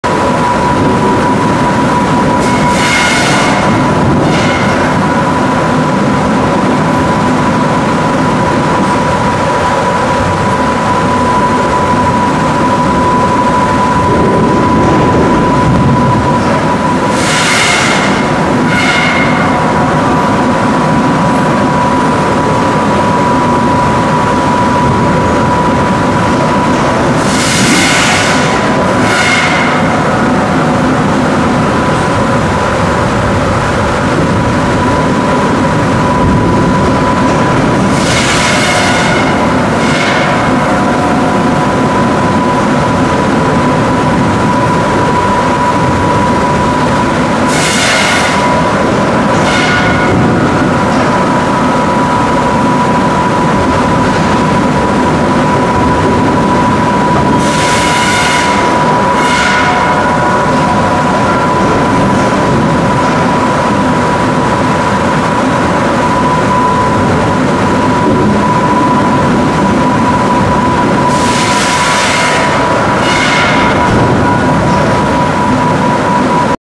Sweep Kicker from V Flight
High Speed Bucking system being fed by a V Flight chain system. The stems are kicked on the fly at speeds up to 500 fpm.